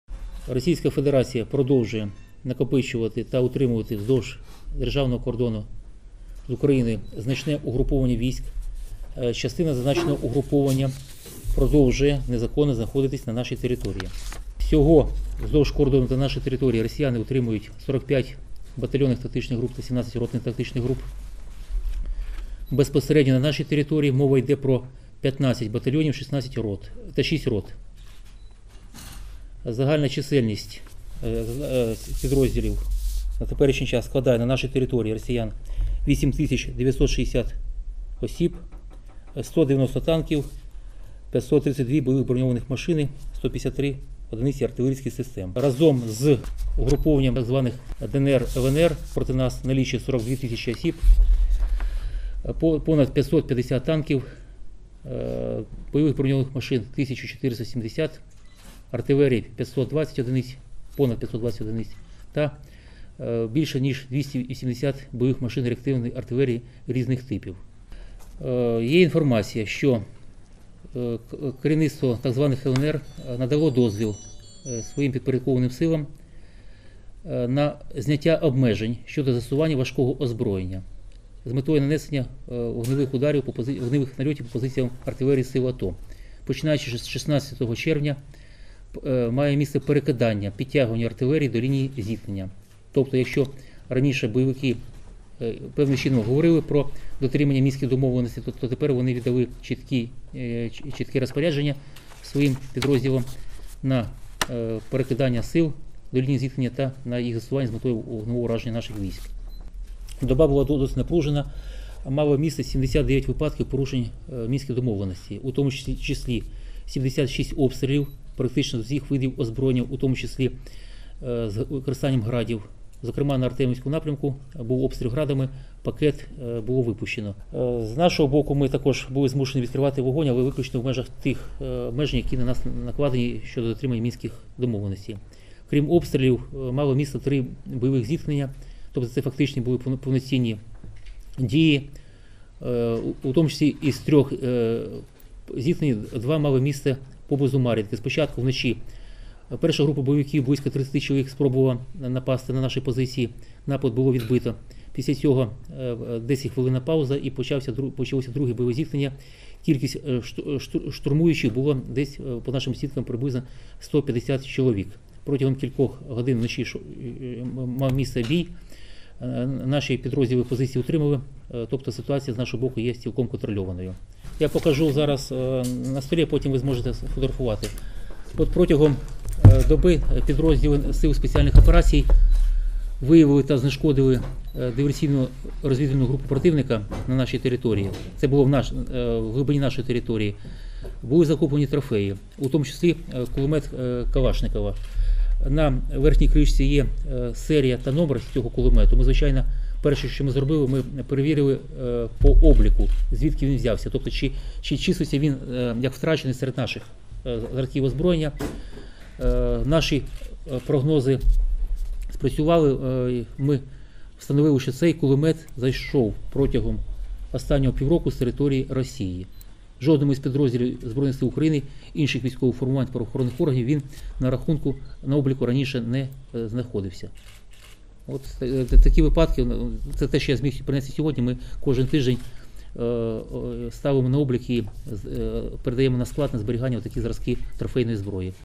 Полковник Галушко продемонстрував трофейний кулемет, завезений із Росії
Автор: Артем Лютий

(Полковник Галушко продемонстрировал трофейный пулемет, завезенный из России)

Заступник керівника АТО, полковник Сергій Галушко, на черговому брифінгу розповів представникам преси про кількість ворожих сил, що знаходяться на окупованій території Донбасу, а також на її кордонах. Він наголосив, що зі сторони бойовиків продовжуються обстріли з крупнокаліберної техніки, у тому числі з «Градів». Українські військові змушені відповідати на обстріли, але в межах тих обмежень, які були накладені Мінськими домовленостями, доповідає полковник. Також Сергій Галушко продемонстрував трофейний кулемет, який було захоплено на контрольованій українськими силами території при знешкодженні ворожої диверсійно-розвідувальної групи. Як стверджує керівництво АТО, кулемет із таким маркуванням не числиться на озброєнні українських військових, його завезли з території Росії.